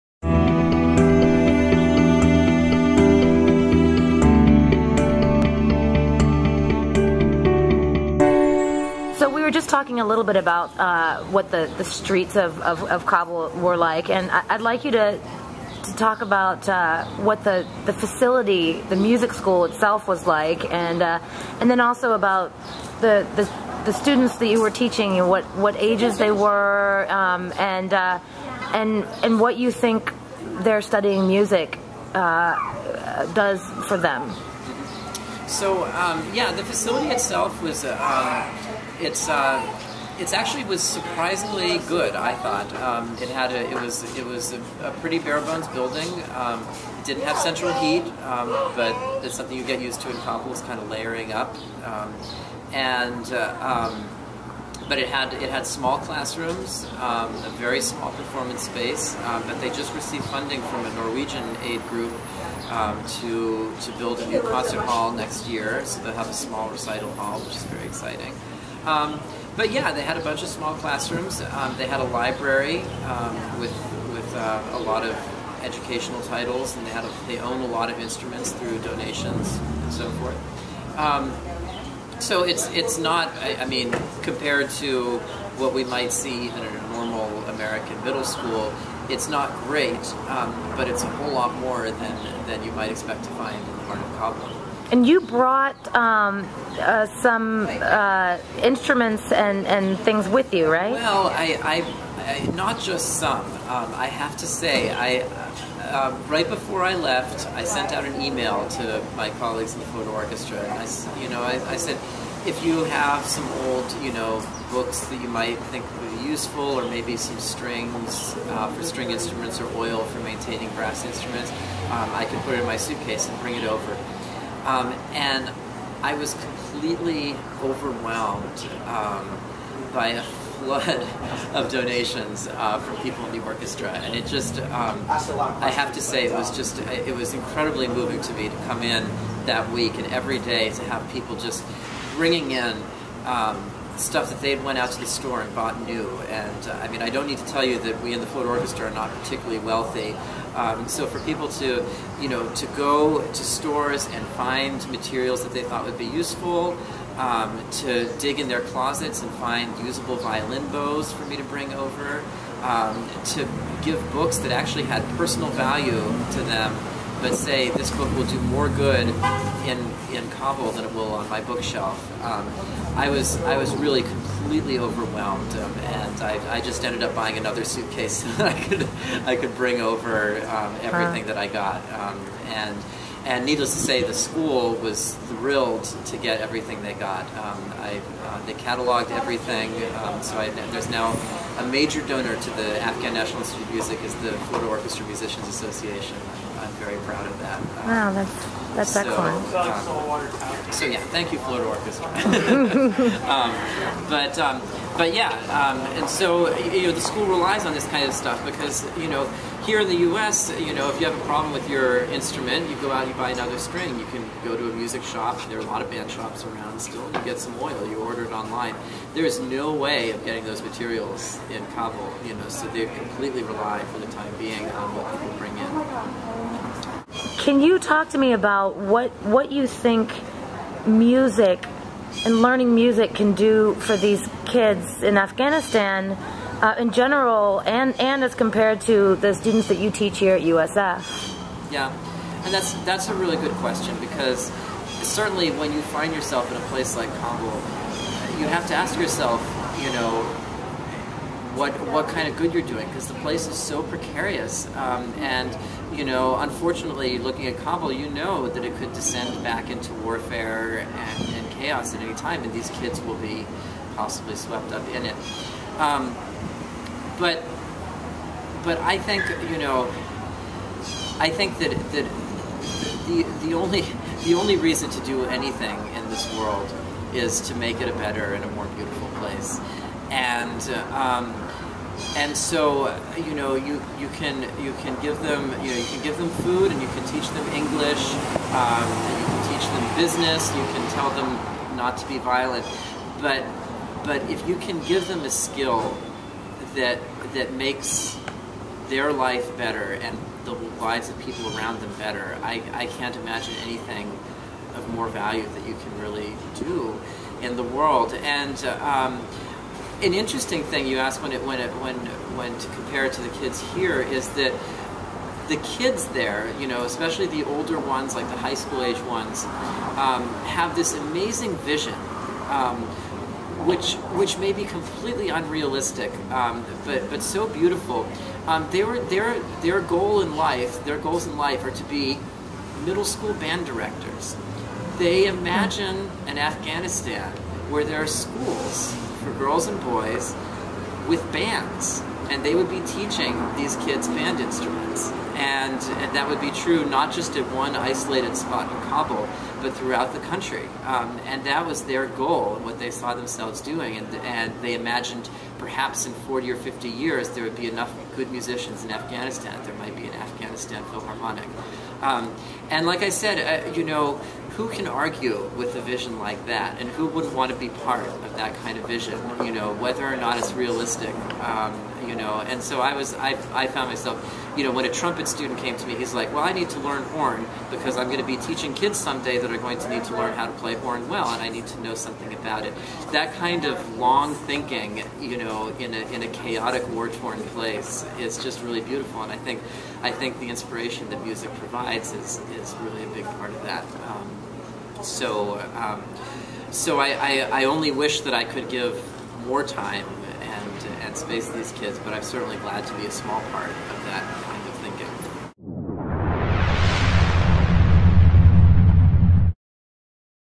Andrew in Afghanistan: the Facilities and the Students
Installment 3 of the interview with french horn player Andrew Karr about his recent teaching excursion to Kabul